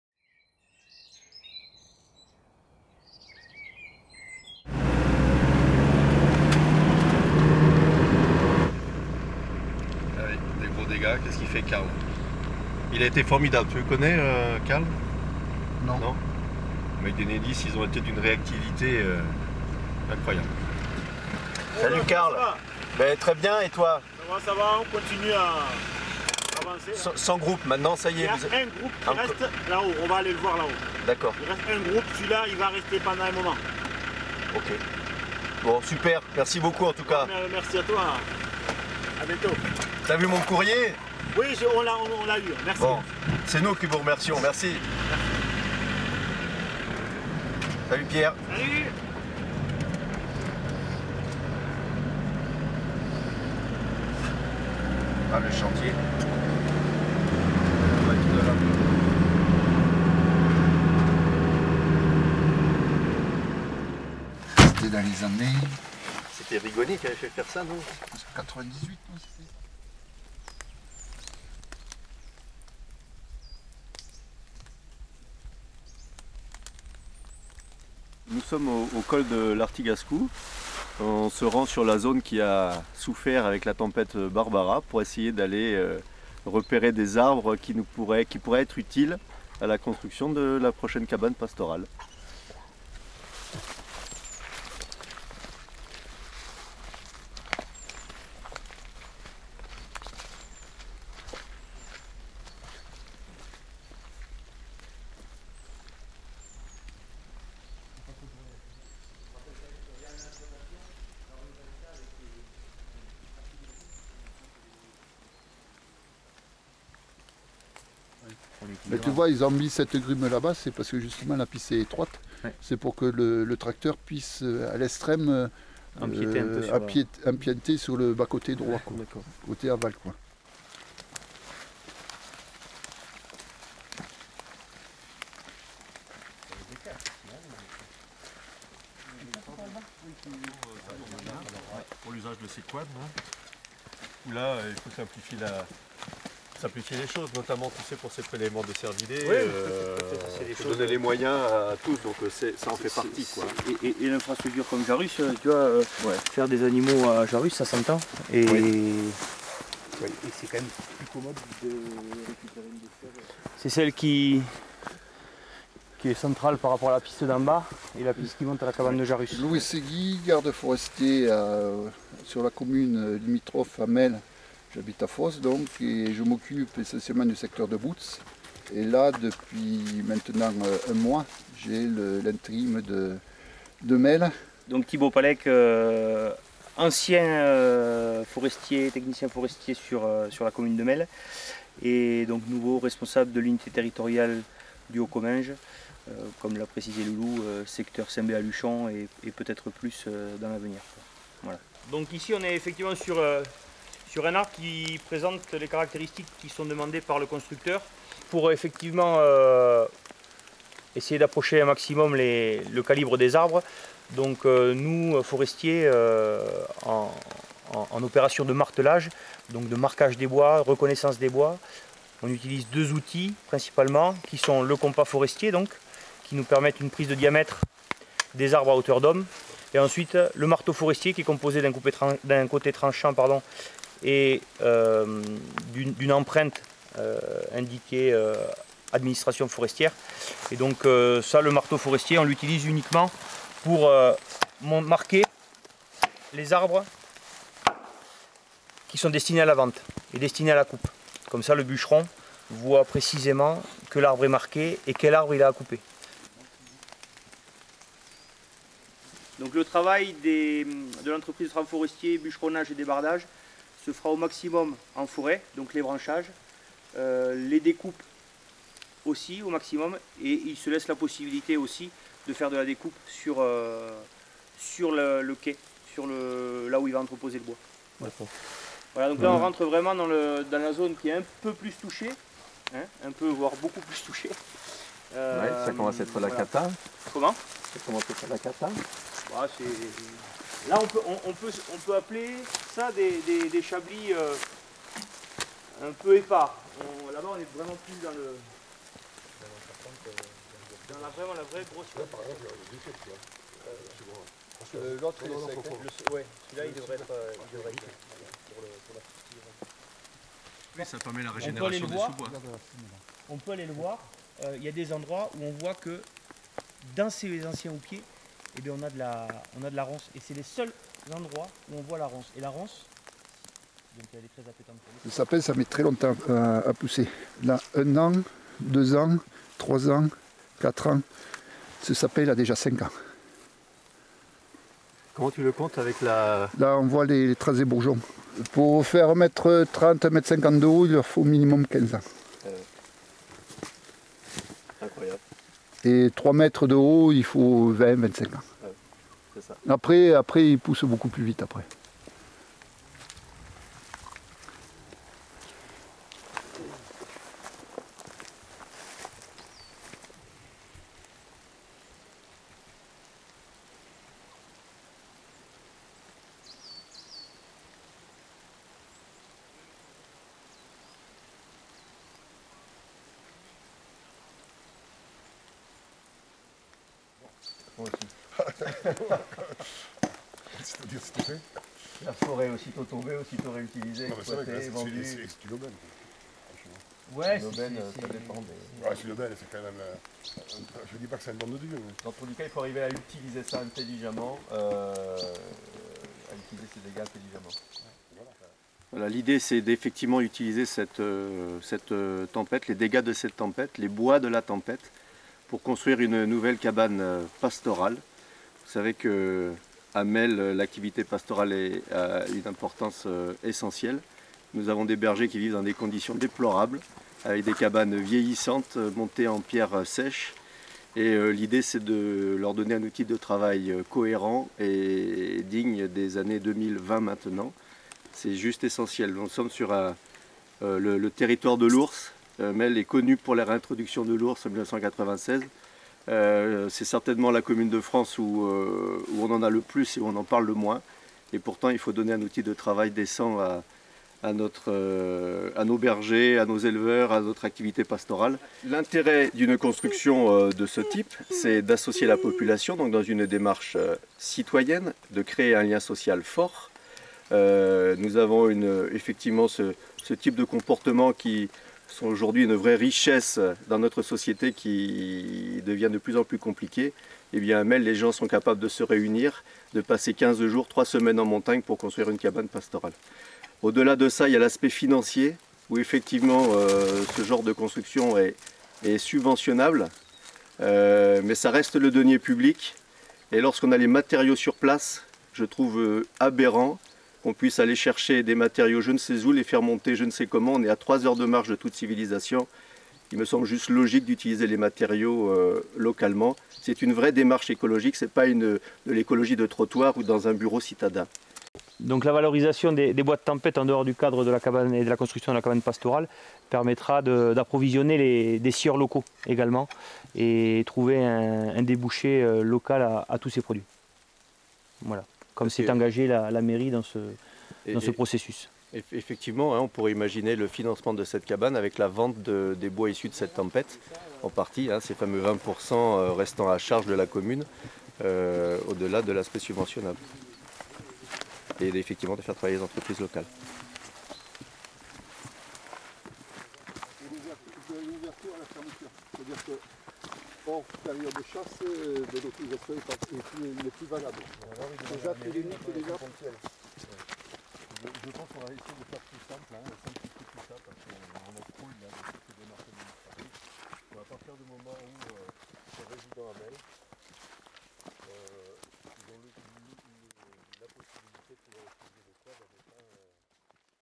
La Cabane
Melles est une commune française située dans le département de la Haute-Garonne, en région Occitanie. Cette vidéo tournée en octobre 2020, présente le projet de construction d'une cabane pastorale en fustes à 1700 mètres d’altitude, au milieu du pays de l’ours, au pied du Pic du Crabère dans les Pyrénées. Le Maire du village, Alban Dubois accompagné de deux gardes forestiers se rendent au col d'Artigascou dans l'idée de réutiliser les arbres déracinés par la tempête Barbara. L'idée est aussi d'impliquer les habitants dans le processus de construction.

oct/nov 2020